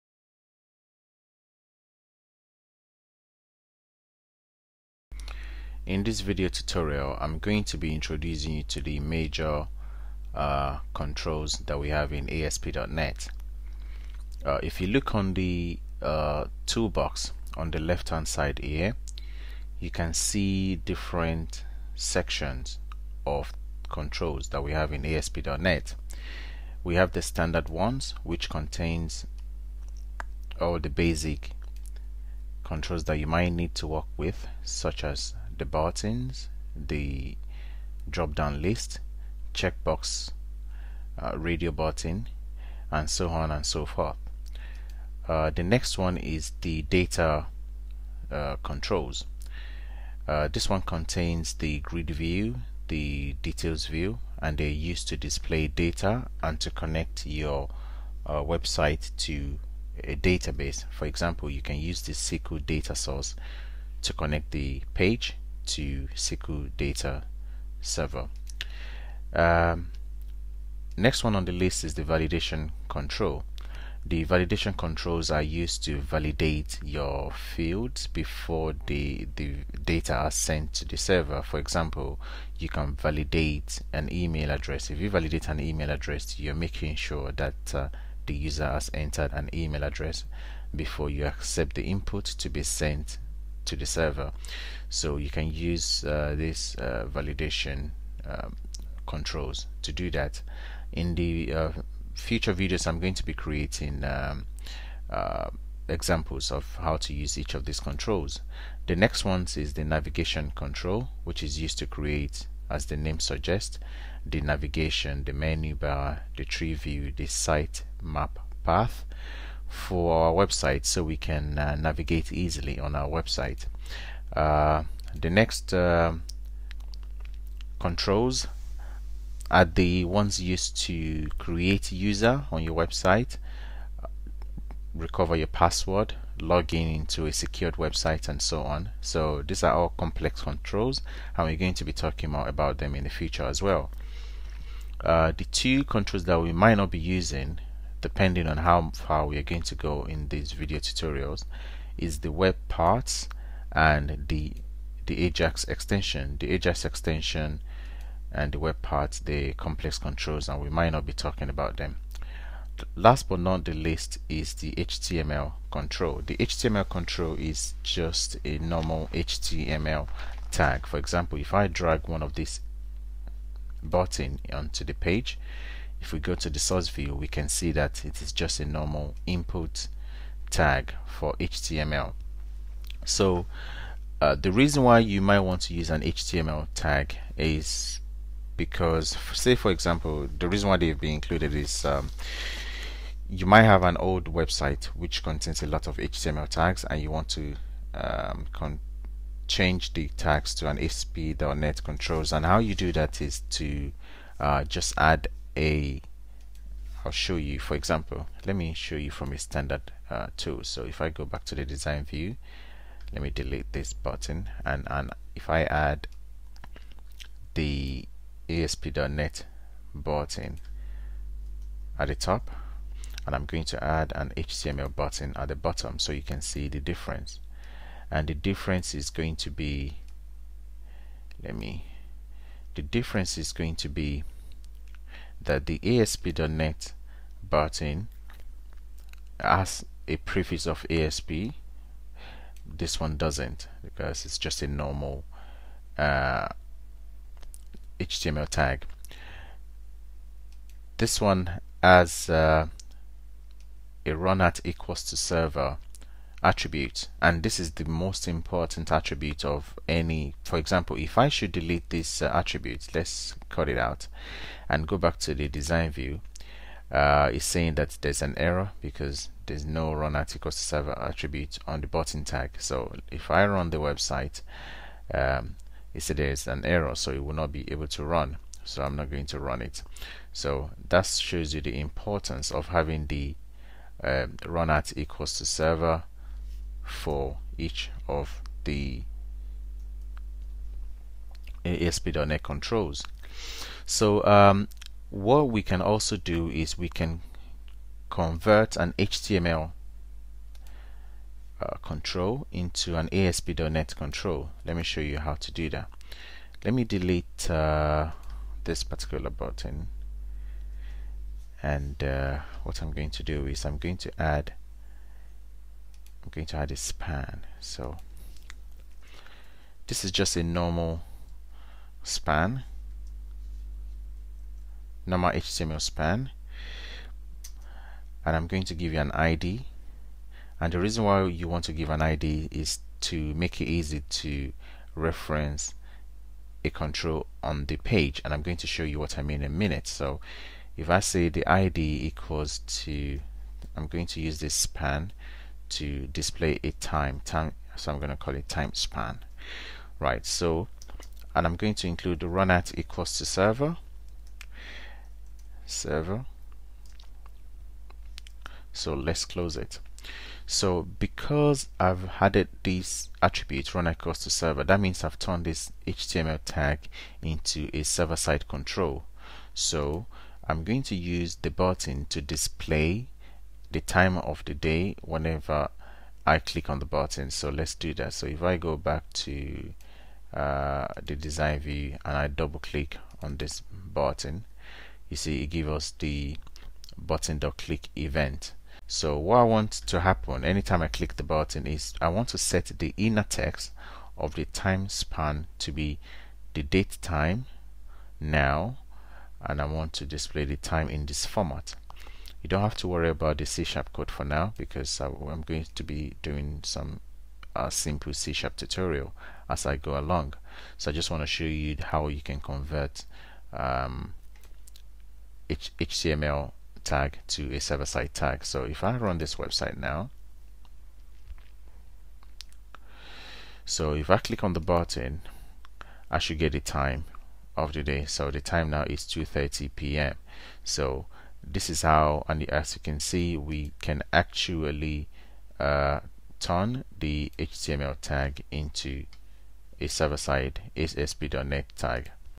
3 - ASP.NET Server and HTML Controls
ASP.NET Controls are divided into different section such as standard controls, data controls, validation controls, navigation controls, login controls, and HTML controls. The HTML control use a normal HTML tag, but are useful because they allow us to take advantage of .NET framework if we add the runat="server" property which converts the HTML control to ASP.NET control.